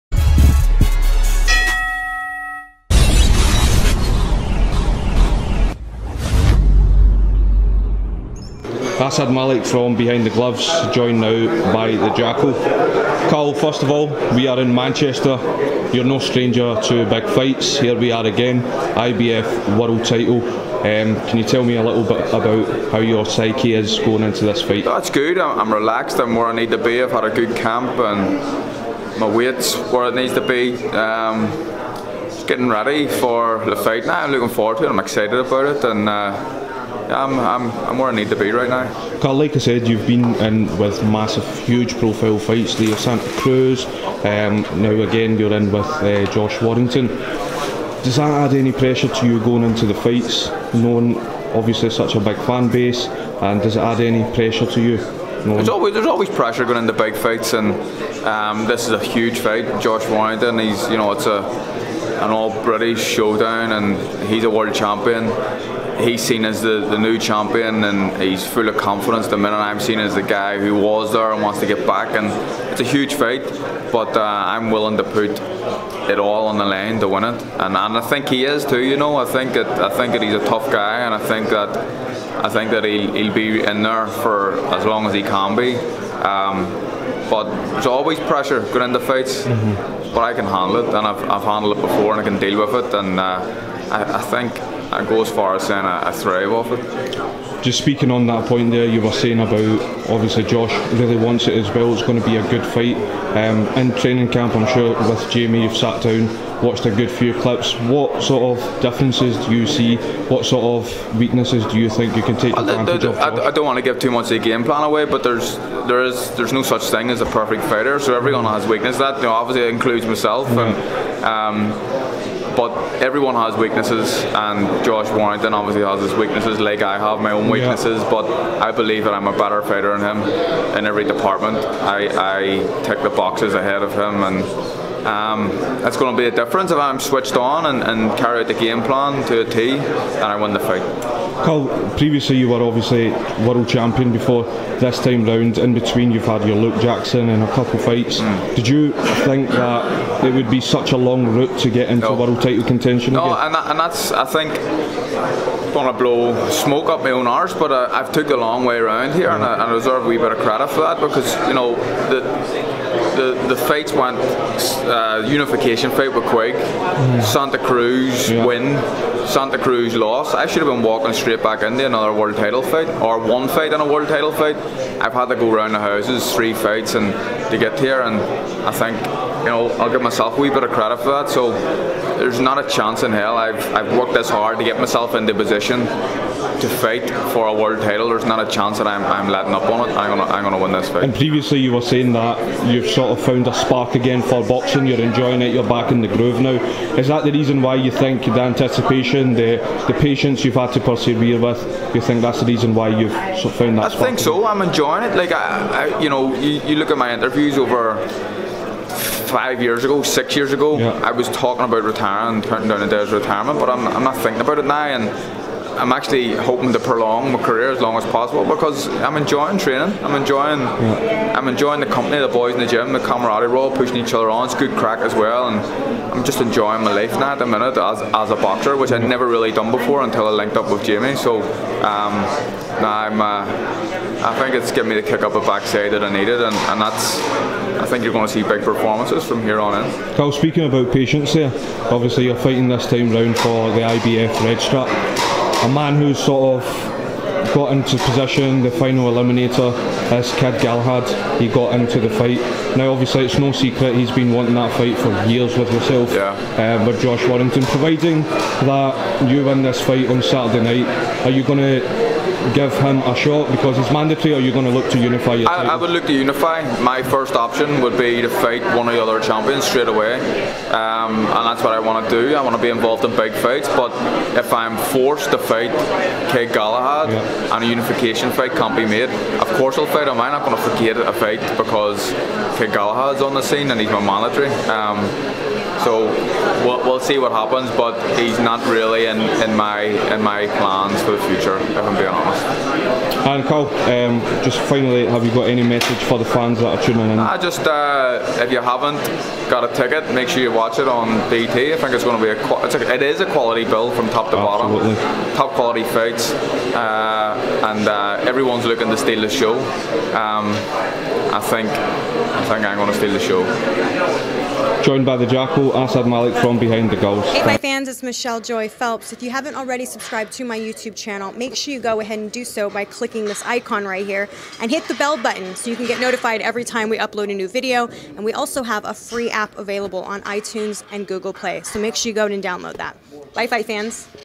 "DONT WANNA BLOW SMOKE UP MY OWN ARSE…I DESERVE BETTER CREDIT”- CARL FRAMPTON ON CAREER + WARRINGTON
Josh Warrington defends his IBF World Featherweight title against Carl Frampton exclusively live on BT Sport Box Office, Saturday 22 December.